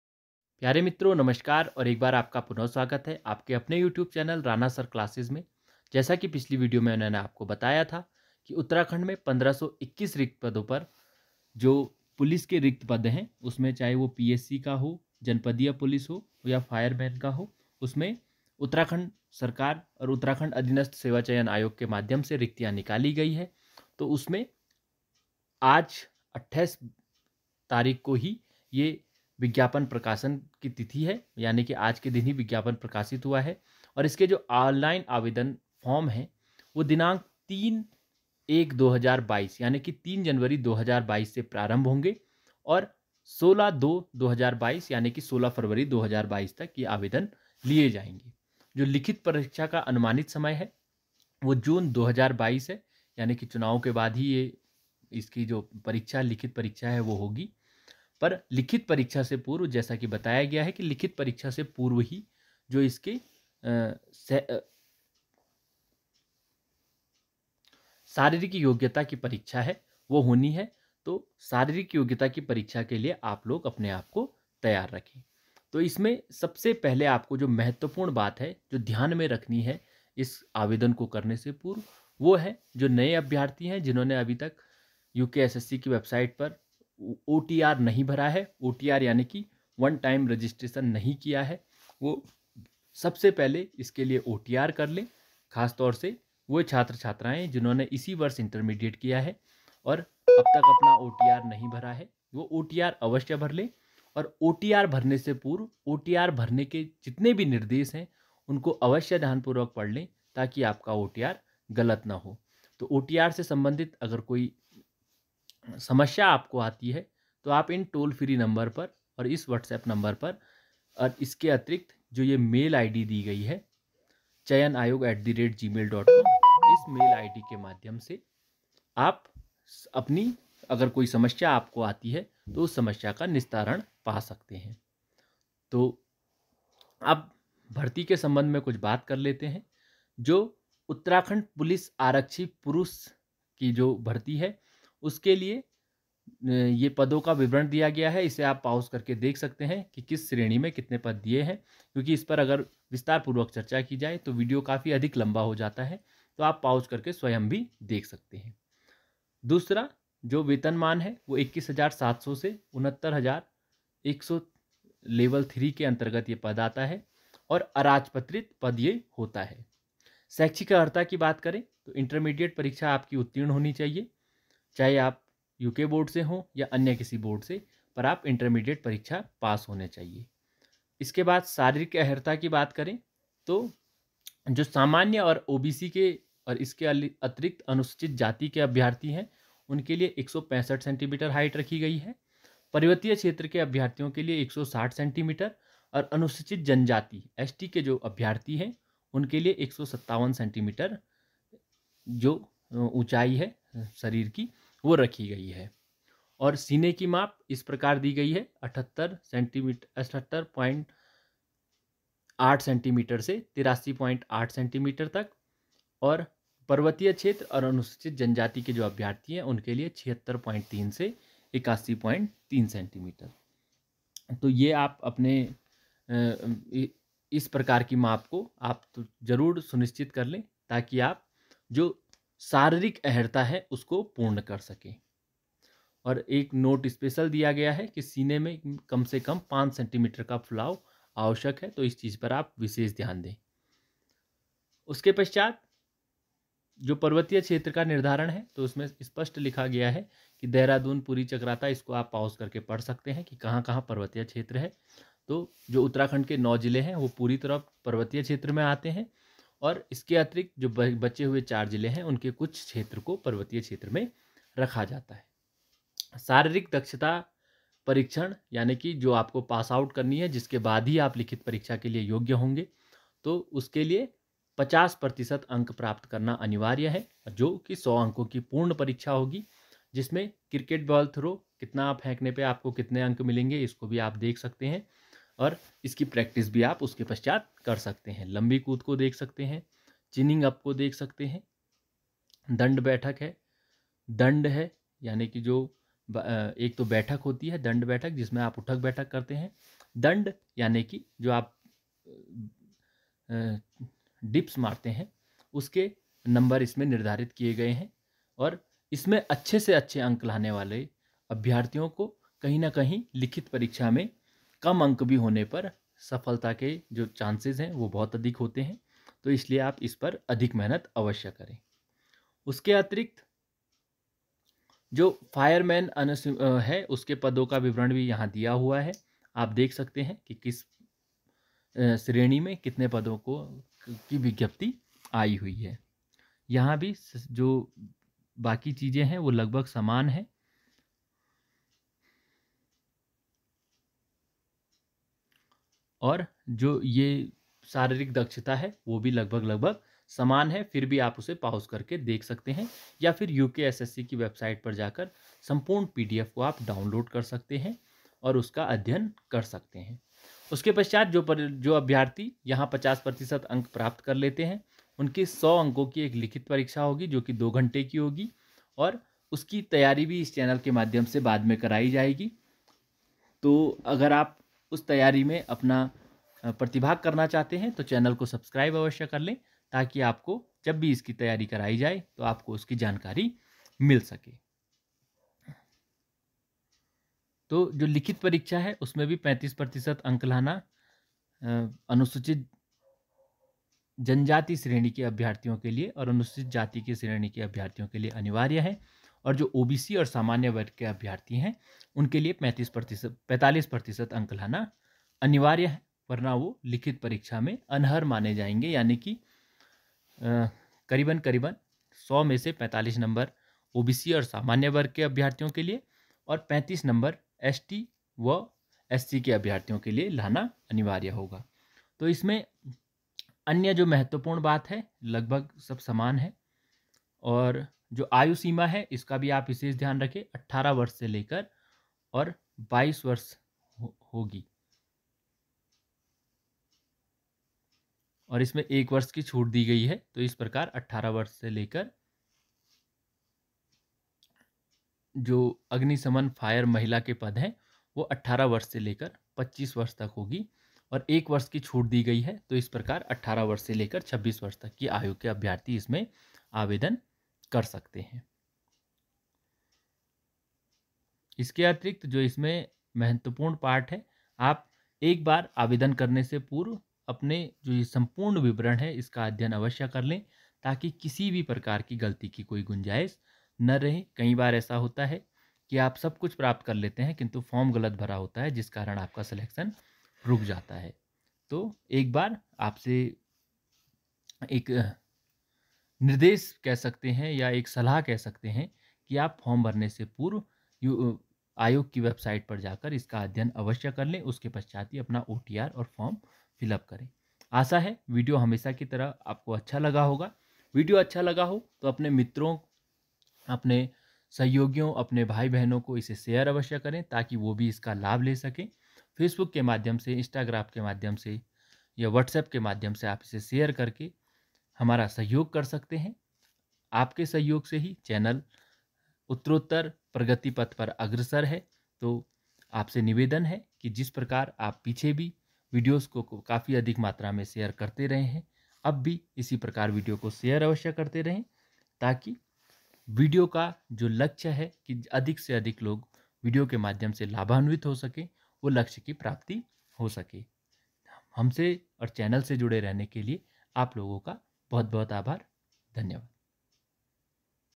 UKSSC POLICE BHARTI उत्तराखंड अधीनस्थ सेवा चयन आयोग ने निकाली पुलिस के पदों पर 1521 भर्ती
join me

Rajendra singh

NTA NET SLET Political science